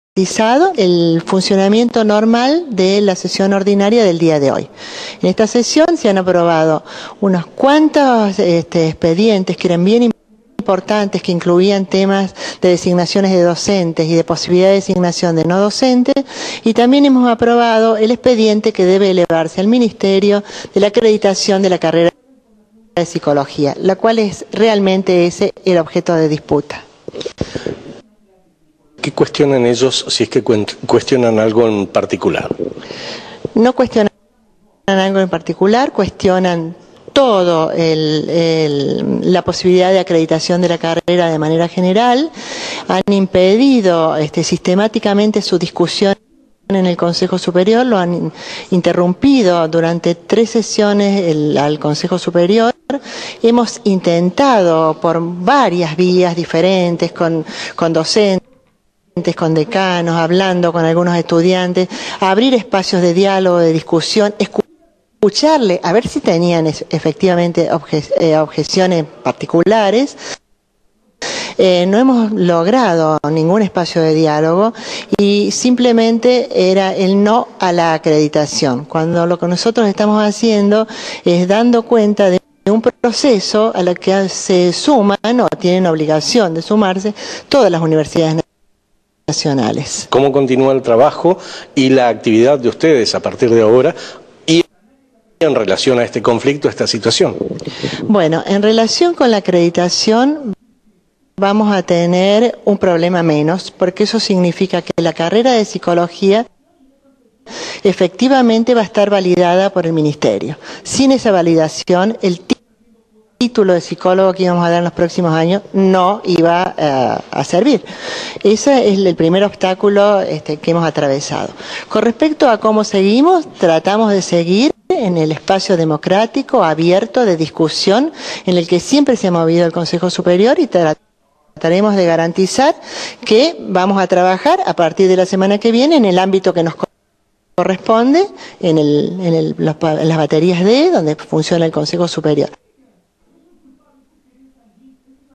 La UNC aprobó cambios en el plan de estudios de Psicología
El Consejo Superior definió algunas modificaciones, tal como exige la CoNEAU para la acreditación de la carrera.

Después de no haber podido sesionar en su sala de reuniones, debido a la irrupción violenta por parte de militantes de algunas agrupaciones estudiantiles, el máximo organismo de la Universidad Nacional de Córdoba aprobó las modificaciones requeridas por la Comisión Nacional de Evaluación y Acreditación Universitaria (CoNEAU) para validar el plan de estudios de la carrera de Psicología.